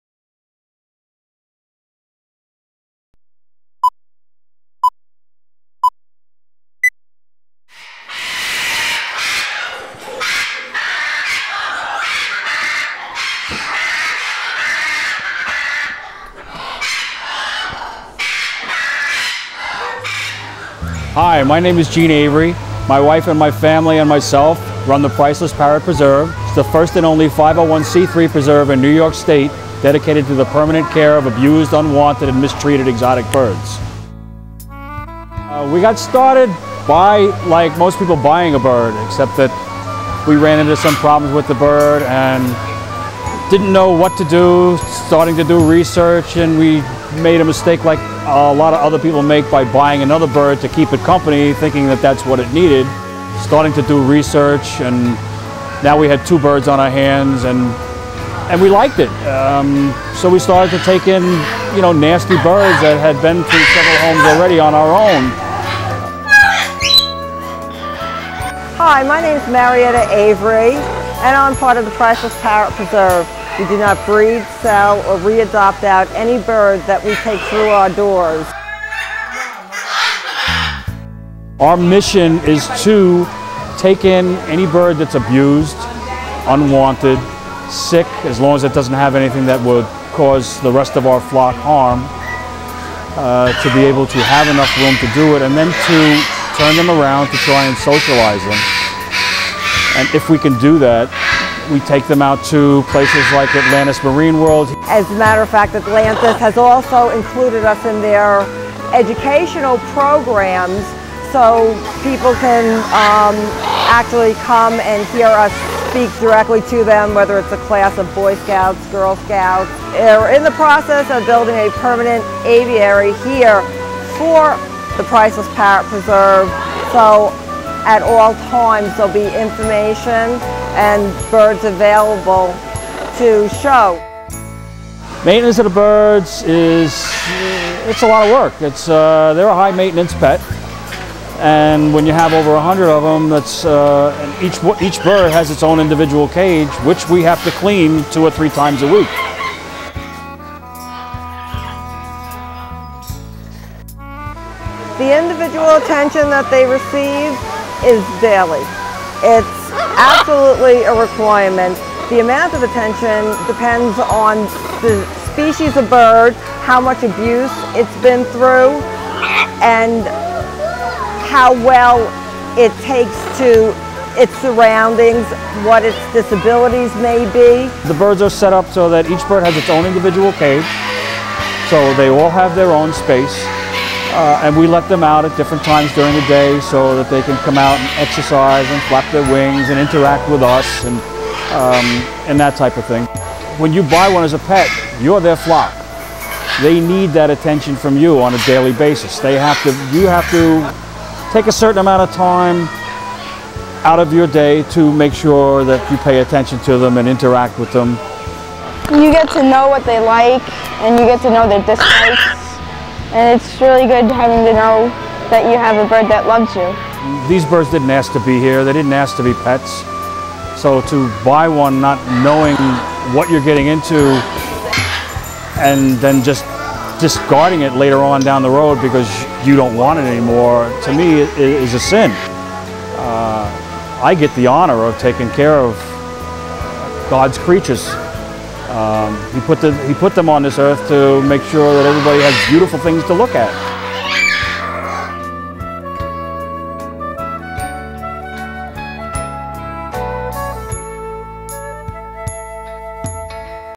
The Priceless Parrot Preserve
Join Gene and Marietta Avery on a tour through the Preserve, and learn about their work rehabilitating abused, sick and aging exotic birds.